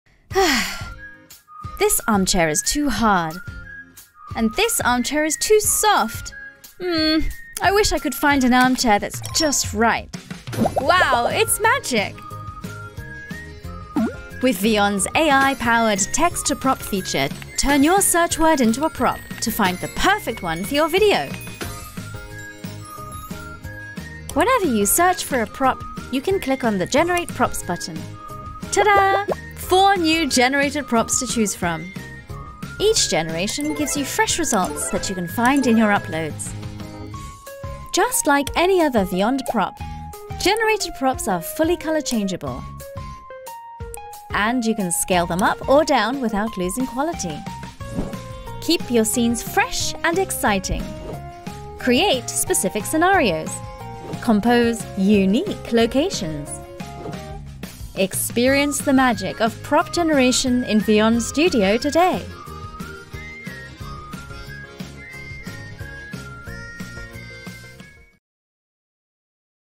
Generate Animated Objects with AI | Vyond Product Features
Find out how quick and easy it is to create your own custom props using Vyond’s AI-powered text-to-prop feature. Quickly personalize any video scenario to make it uniquely yours. Long story short, the possibilities are endless.